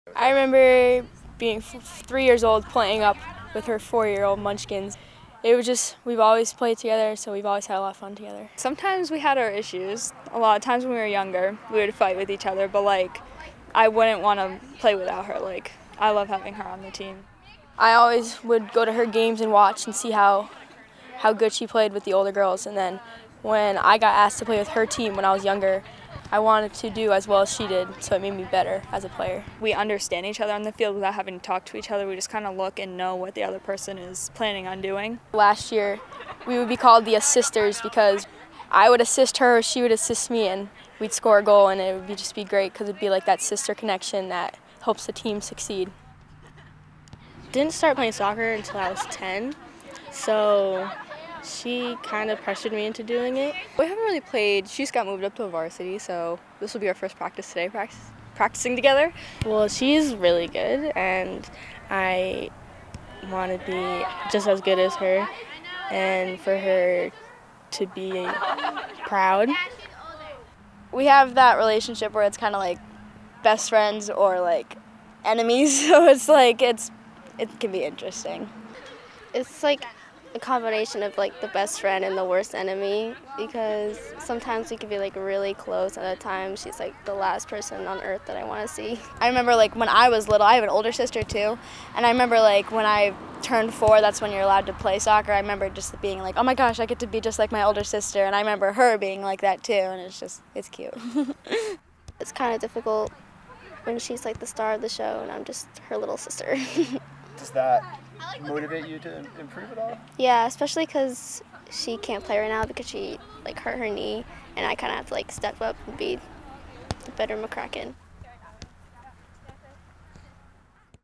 Montville's soccer sisters
Three pairs of sisters on the Montville girls' soccer team - Maddie and Kara Beaupre, Michaela and Kiera MacCracken, and Olivia and Alexis Meehan - have formed bonds that transcend sibling rivalry.

Listen live Monday at 6:30 as Montville hosts St. Bernard